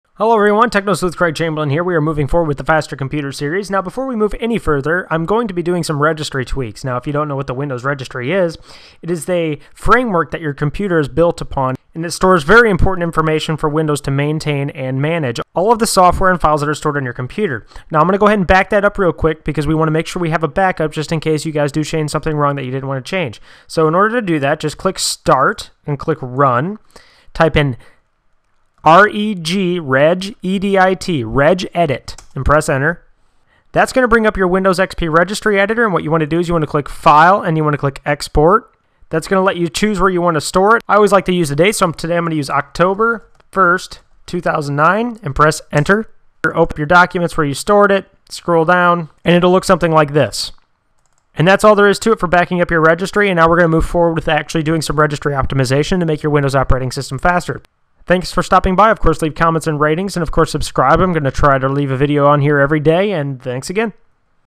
How To Backup The Registry On Windows XP
This tutorial will show how to backup registry on Windows XP.